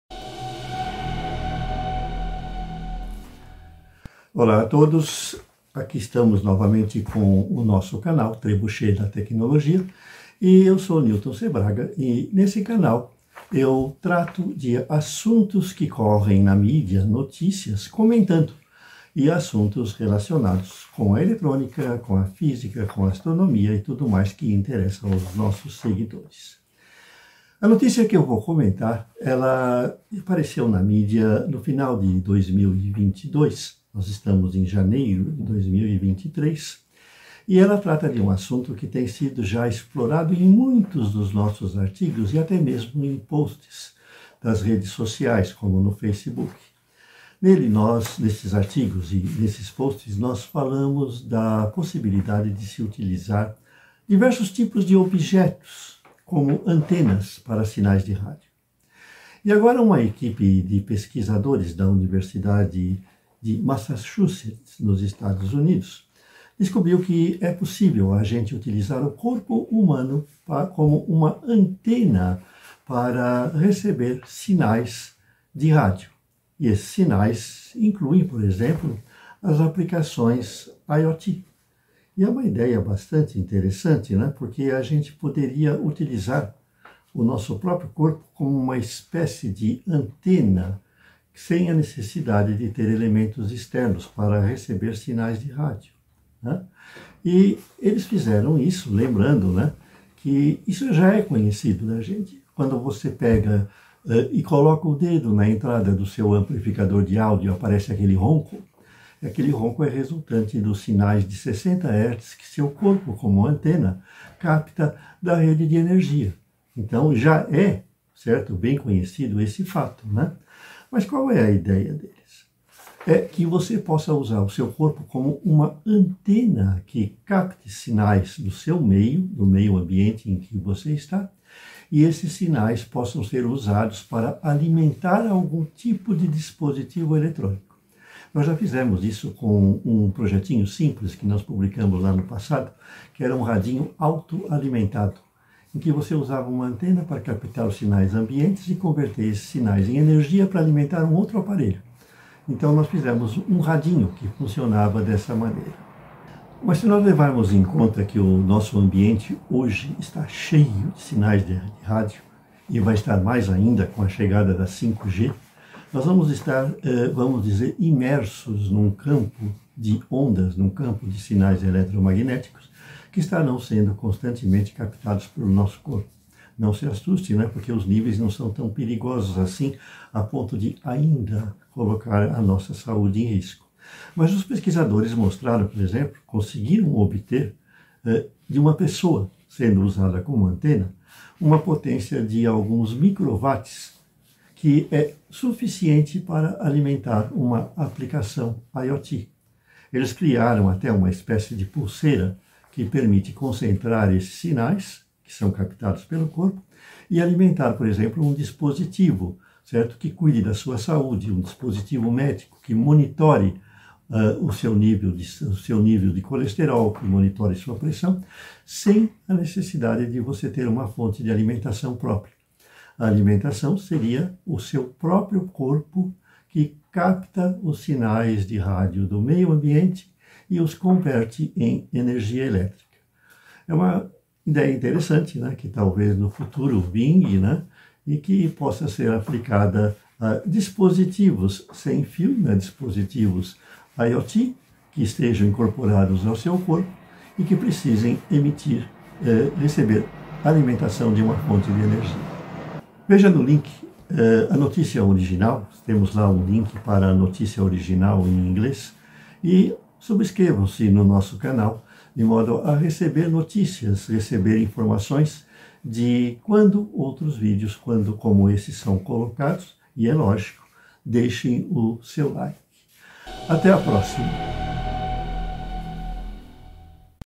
Corpo Humano como Antena (VTR139)
Janeiro de 2022 – Em recente post que colocamos nas nossas mídias falamos da possibilidade de se usar qualquer objeto condutor como uma antena de rádio. Até exploramos uma ideia de 1932 em um antigo rádio de carro usava uma cerca de fazenda para servir de antena e assim captar estações mais distantes. Mas a notícia que vimos agora mostra que podemos ir além e até usar nosso próprio corpo como antena com as tecnologias do 5G e quem sabe outras. Neste vídeo comentamos a ideia que vem da universidade de Massachusetts nos Estados Unidos, onde pesquisadores encontraram um meio de concentrar a energia de RF do ambiente, numa ampla faixa do espectro, de modo a utilizá-la para alimentar dispositivos vestíveis. Os pesquisadores criaram então um bracelete que pode colher alguns micro-watts de energia o que é suficiente para alimentar vestíveis de baixo consumo como, por exemplo, um monitor cardíaco.

     Links:
Para notícia original: